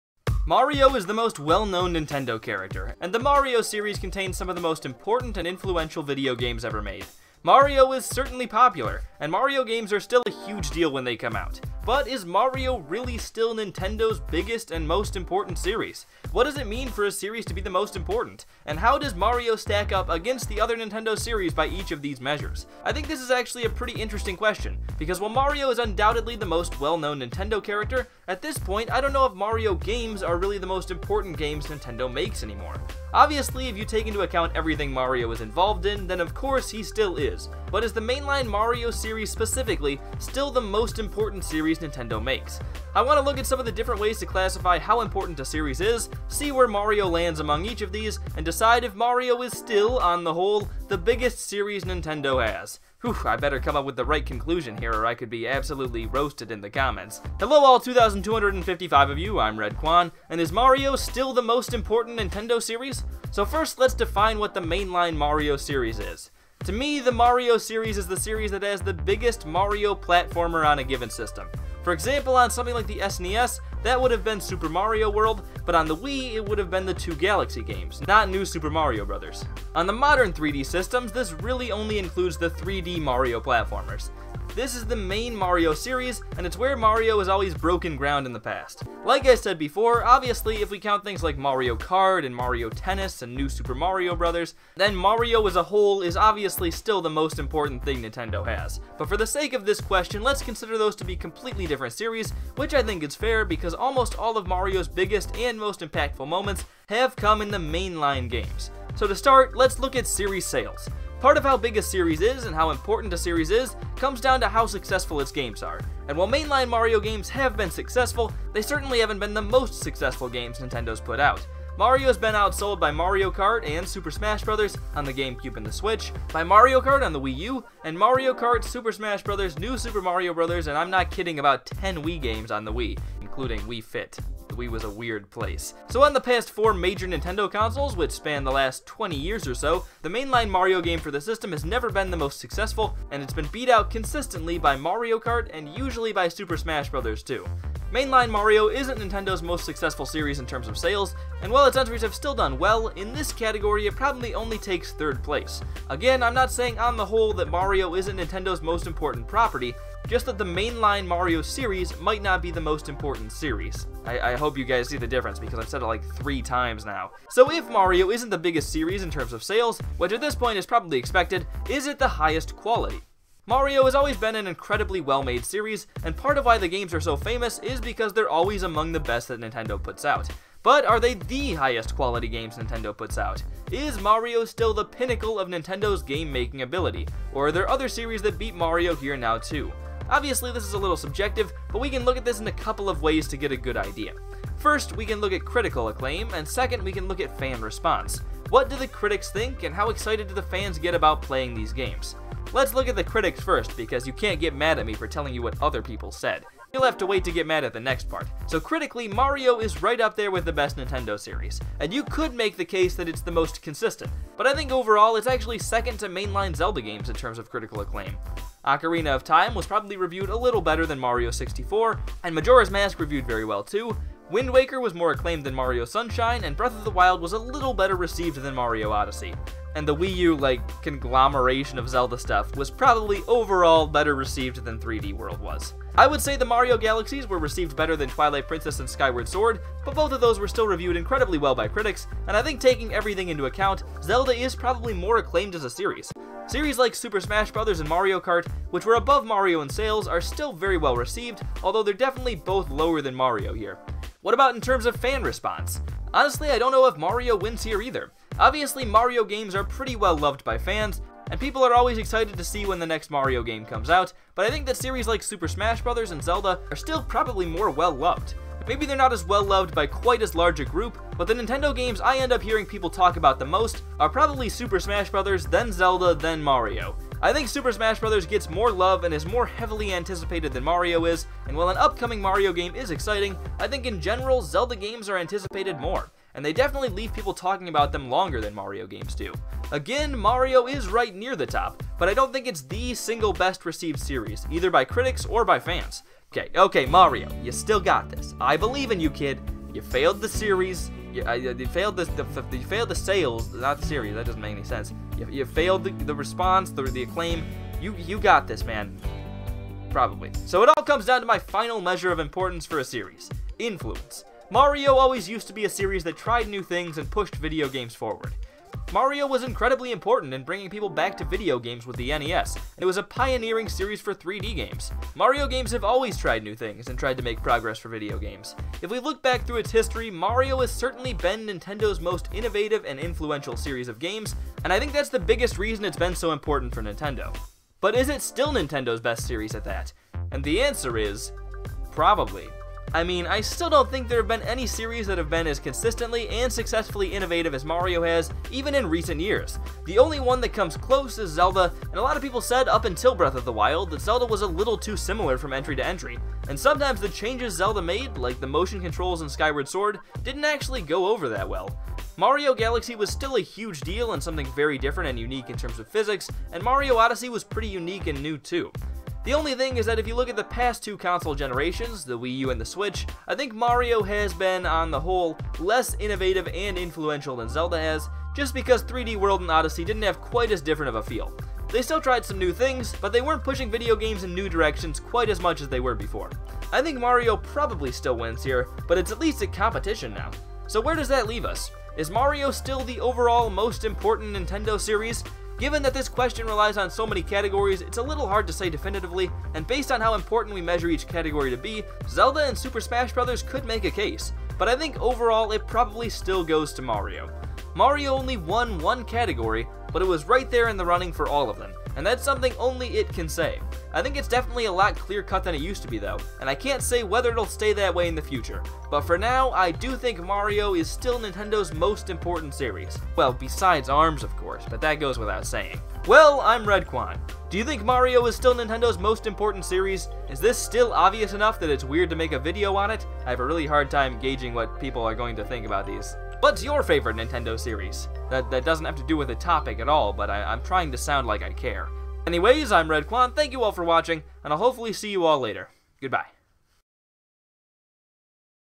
Is Mario Still Nintendo's Biggest Series?
Mario has always been Nintendo's biggest series. It's the series that people associate with Nintendo, it's the series that's always been the face of Nintendo's brand. But is Mario still the series that is most important for Nintendo? This internet loser thinks there's a possibility it isn't.